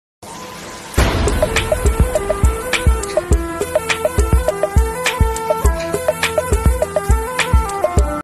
The HungryTigers Climbing Tree For Prey
The best place to see Tigers In India is kanha national park.
You may bw aurprused thT tiger can climb tree  like a cat.